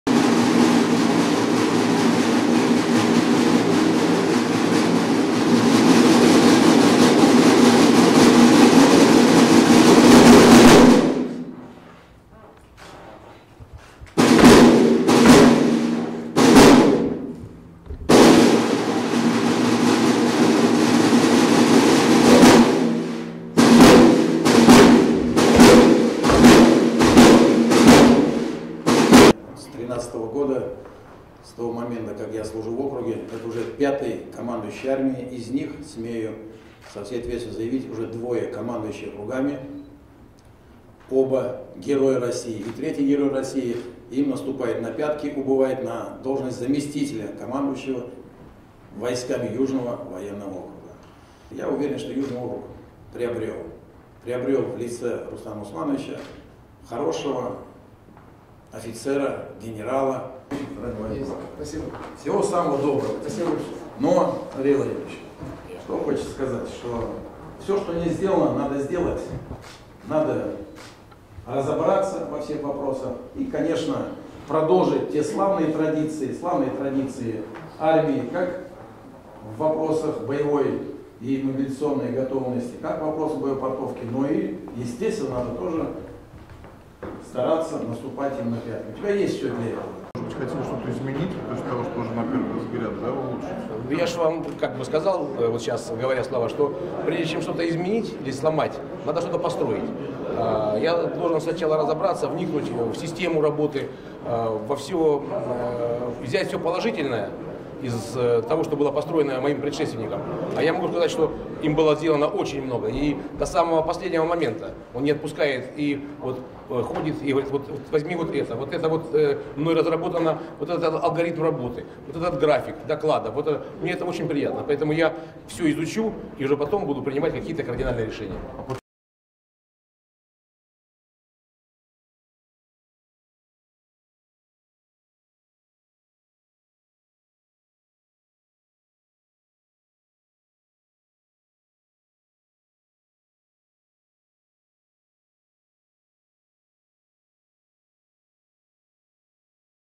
Личный штандарт командарма! Андрей Колотовкин возглавил самарскую общевойсковую армию
Генерал-майор Андрей Колотовкин назначен командующим 2-й гвардейской общевойсковой армией, дислоцированной в Поволжье. Начальник штаба Центрального военного округа генерал-полковник Евгений Устинов на церемонии в Самаре вручил ему личный штандарт и представил офицерам.
Колотовкин родился 10 апреля 1972 года в Майкопе. Окончил Санкт-Петербургское высшее общевойсковое командное училище (1994), Общевойсковую академию (2006), Военную академию Генерального штаба (2015). Прошел путь от командира мотострелкового взвода до первого заместителя командующего общевойсковым объединением Южного военного округа.
Награжден орденом Александра Невского (2016), двумя орденами Мужества (1995, 1997), орденом «За военные заслуги», медалью «За отвагу».